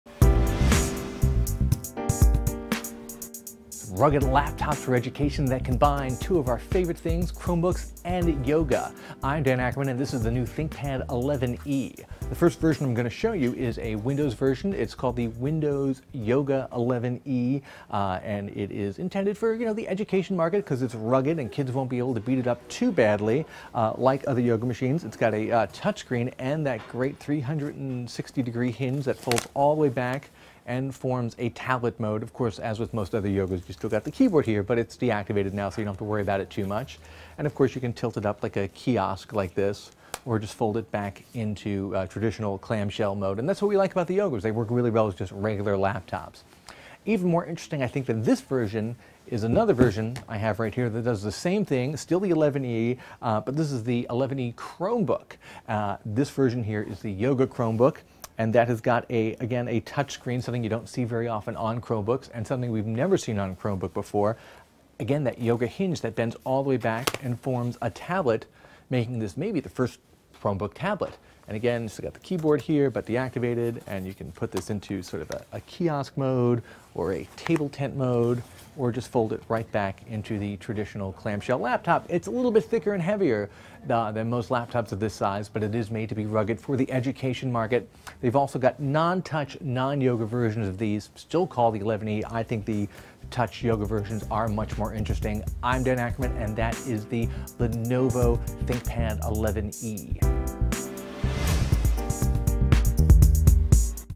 Hands on with the Lenovo Thinkpad 11e series
Students get four 11-inch choices, Windows and Chrome OS, in both touch and non-touch.